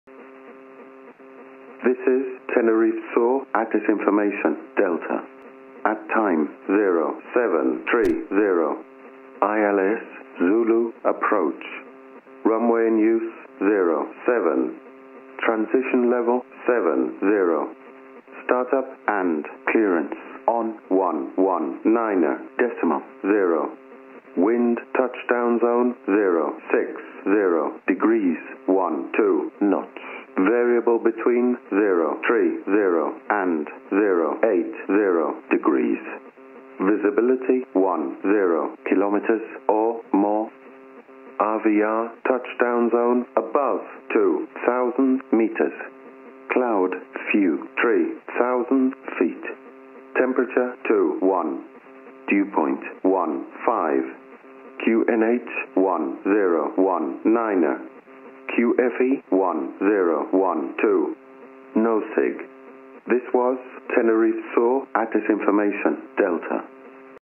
ATIS or Automatic Terminal Information Service.
ATIS or Automatic Terminal Information Service. We usually listen to the information before contacting the ATC. 🔊